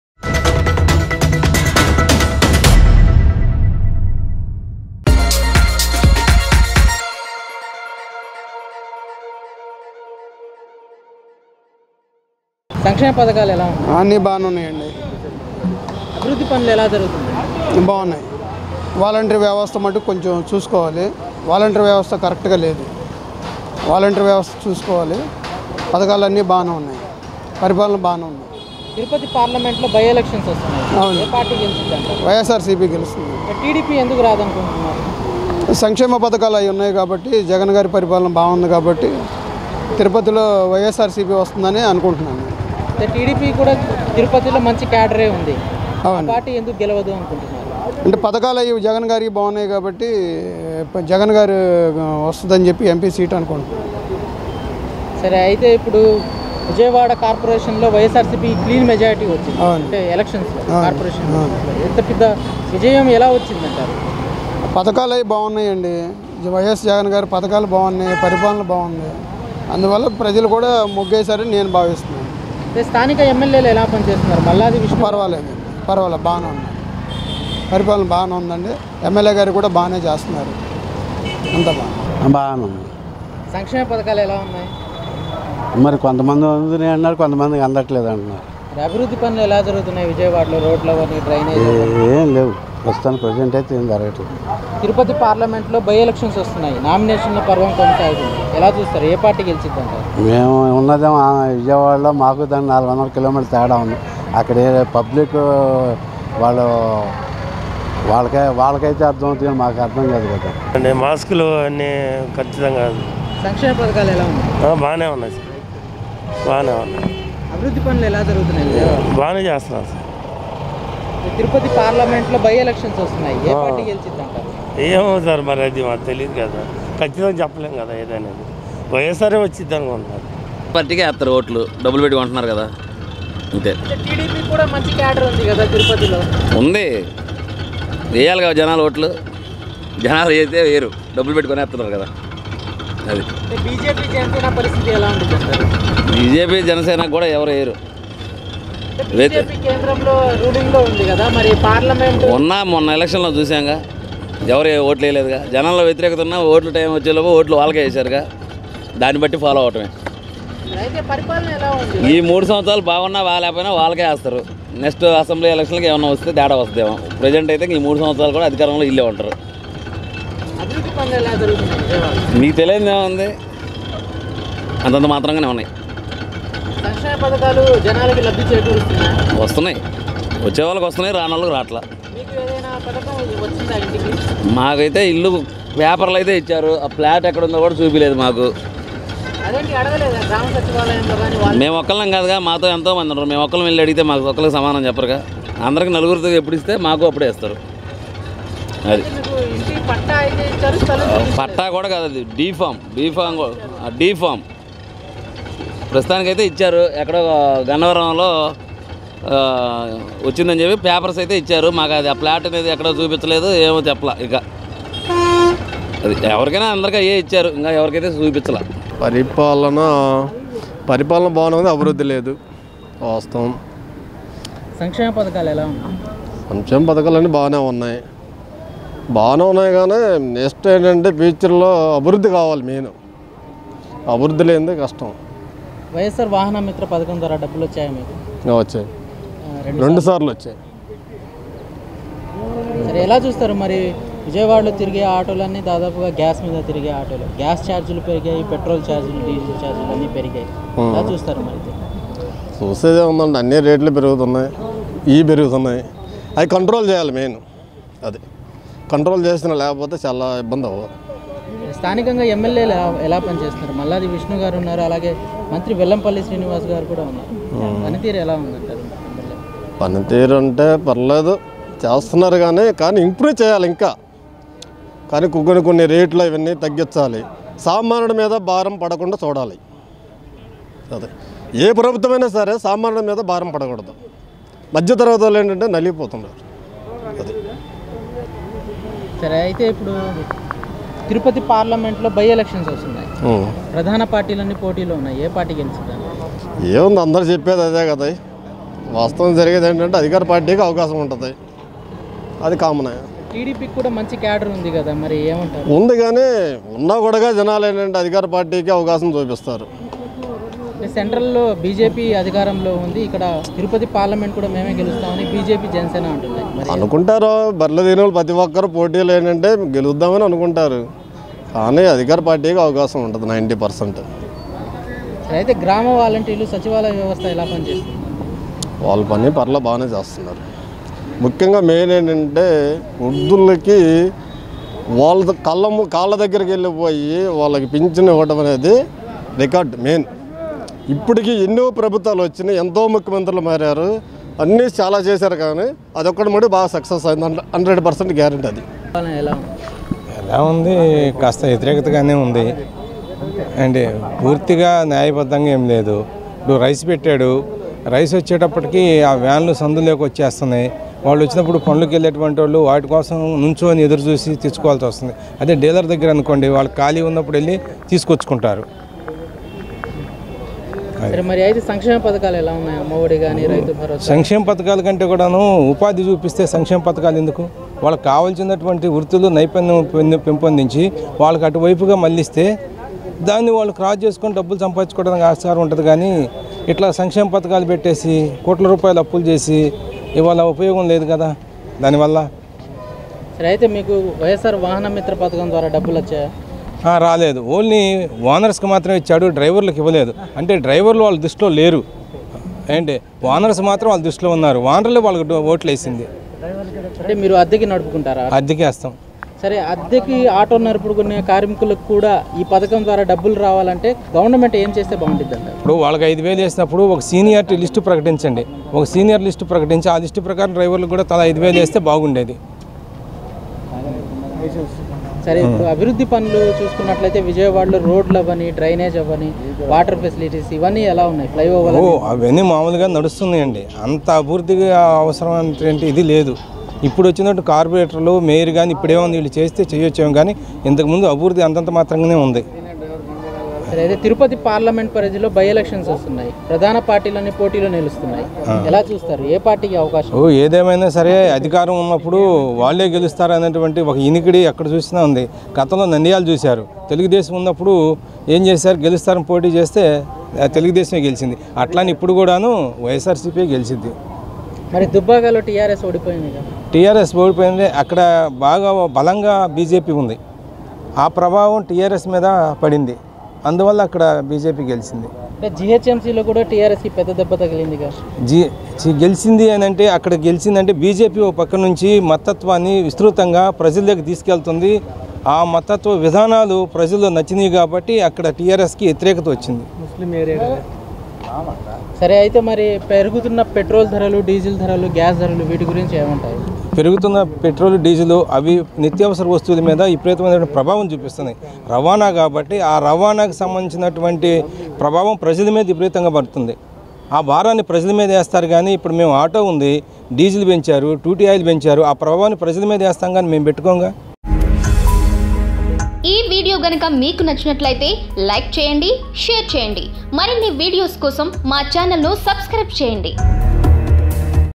Public Talk | YS Jagan | తిరుపతిలో గెలుపు బాటలో ఎవరున్నారో ... ప్రజల మాటల్లో | PDTV News
తిరుపతిలో గెలుపు బాటలో ఎవరున్నారో. ప్రజల మాటల్లో